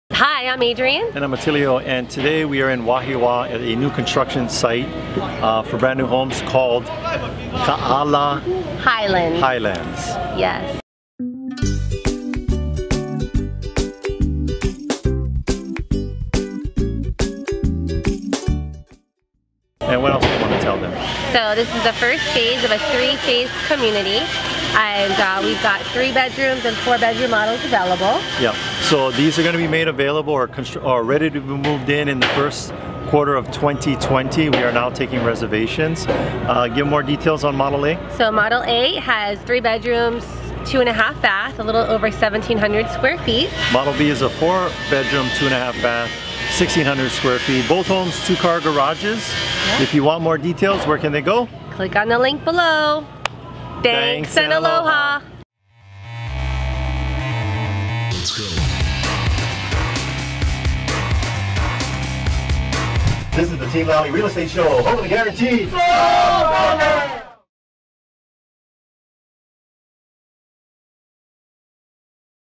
Have You Heard of Kaala Highlands?
We’re coming to you today from a new construction site called Kaala Highlands. This is the first phase of a three-phase community, and there are 3-bedroom and 4-bedroom models available to move into during the first quarter of 2020. We’re currently taking reservations for all models. This would be a beautiful place to live. To see the construction of the models for yourself, watch this short video.

If we can't get your home sold we will buy it!

Team Lally
Keller Williams Honolulu